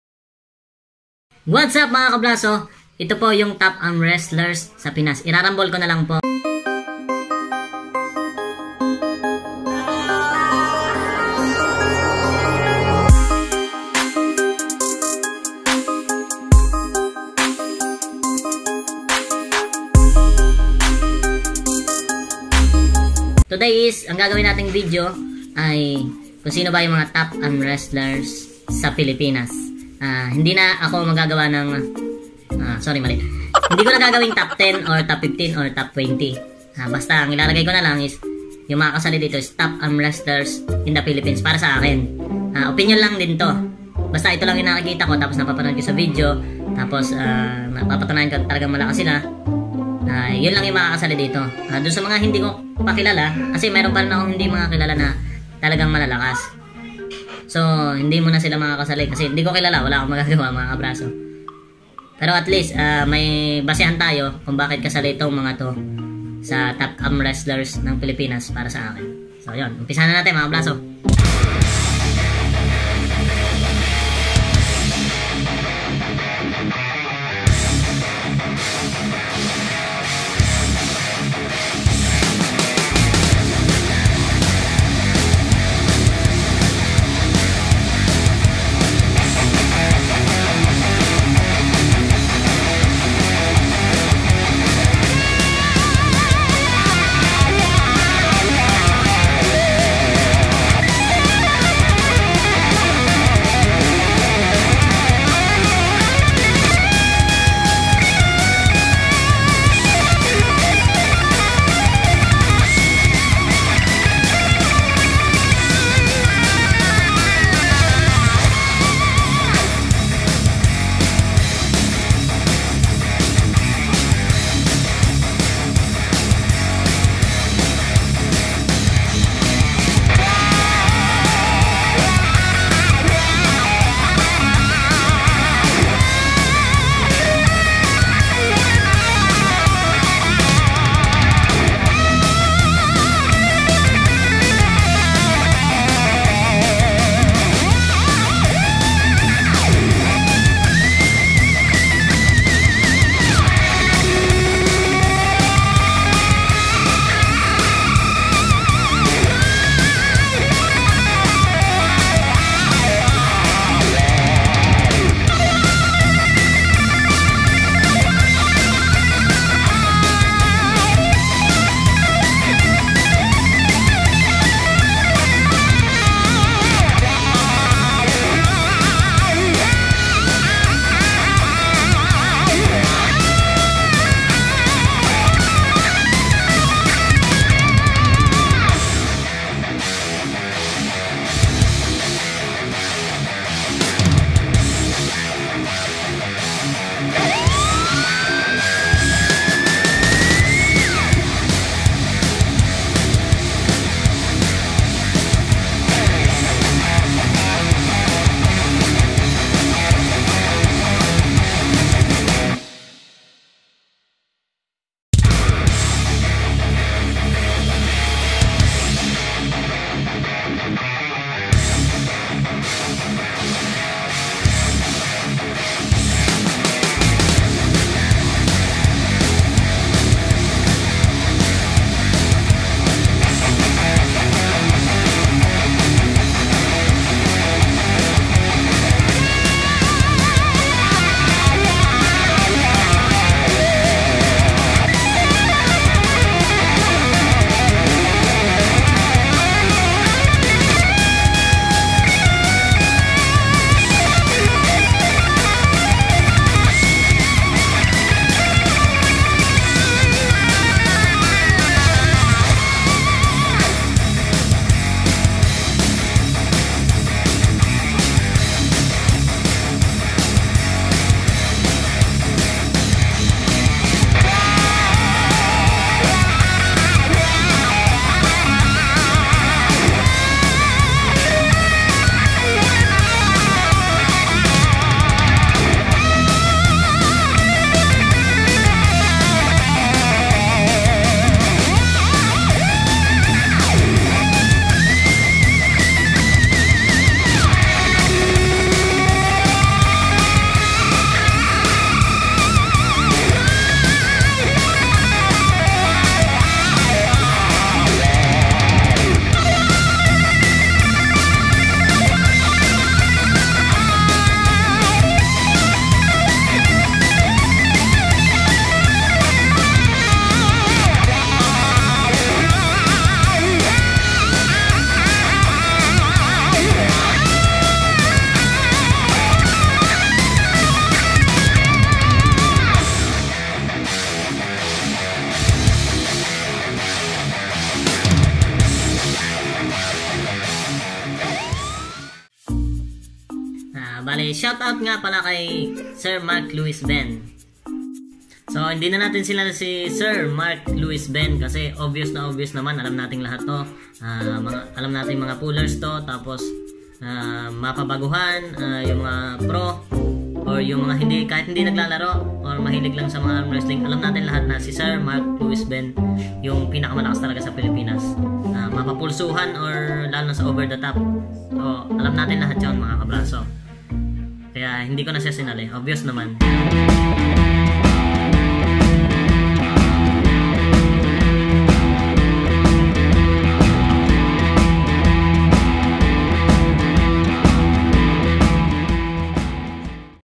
Philippines famous armwrestlers | Pulsuhan | over the top
Mga kabraso ito po ang mga kilala at talagang malalakas na armwrestlers or pullers dito sa Pinas mapa Pulsuhan or over the top. Pasensya na po dun sa ibang malalakas na hindi natin kilala kaya hindi po naisama dito sa ginawa nating video. Maraming salamat po sainyo mga kabraso. Godbless